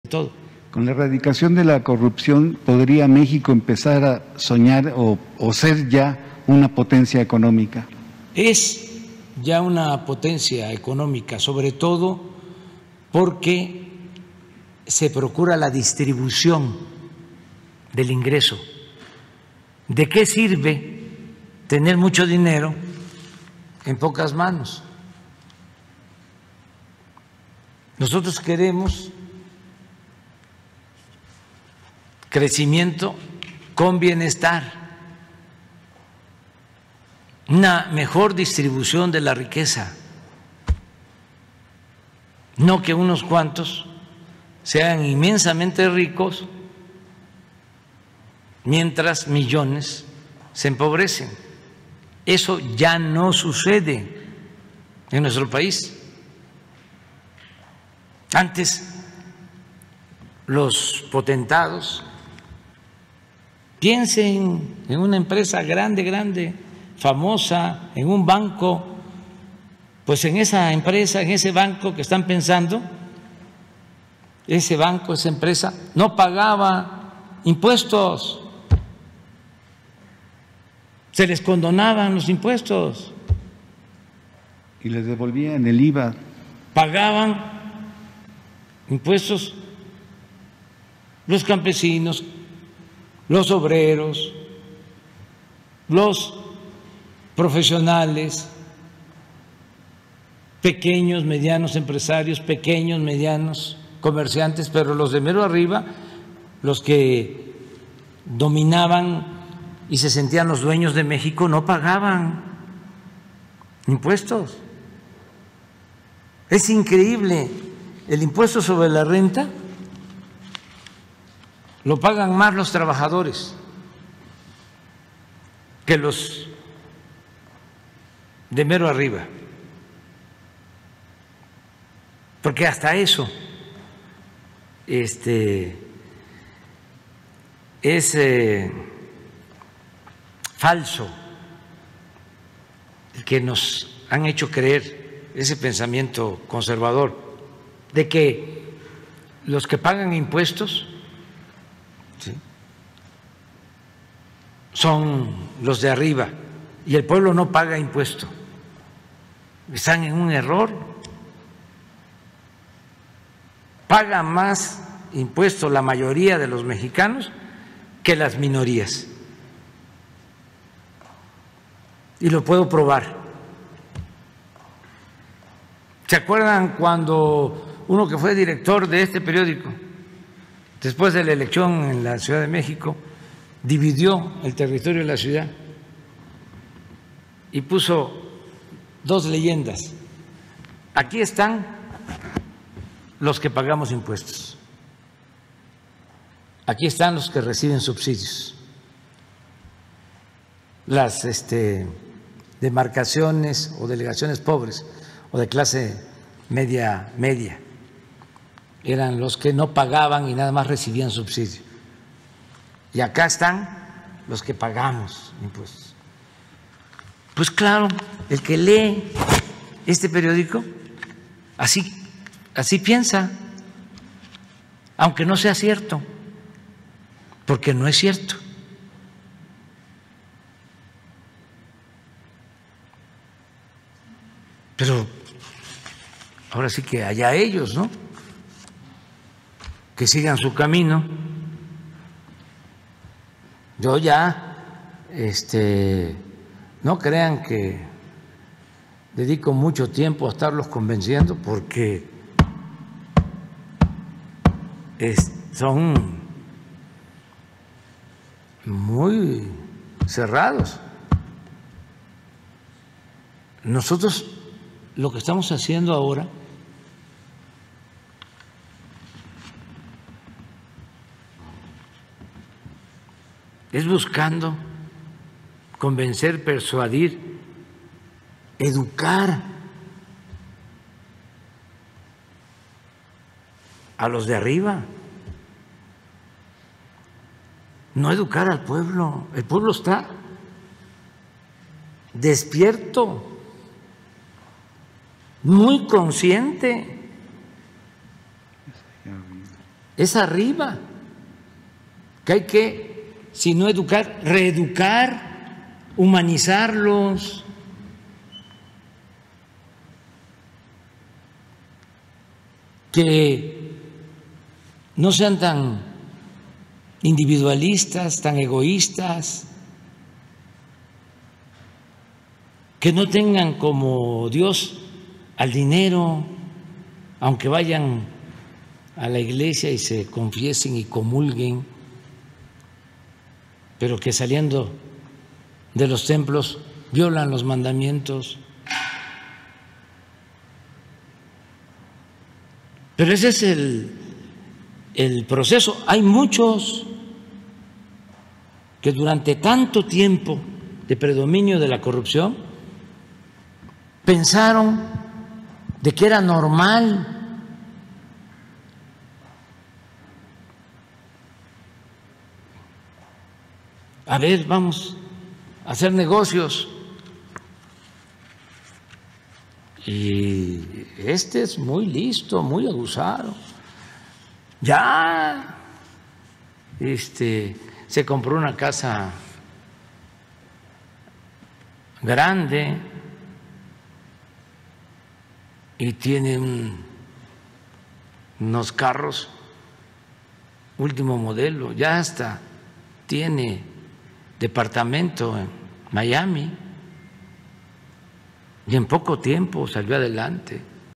AMLO ACLARA QUE MÉXICO ES YA UNA POTENCIA PORQUE SE PROCURA LA DISTRIBUCIÓN DE LA RIQUEZA
El presidente López Obrador aseguró que México es ya una potencia porque se procura la distribución de la riqueza, cuando antes se le perdonaban los impuestos a los pobres.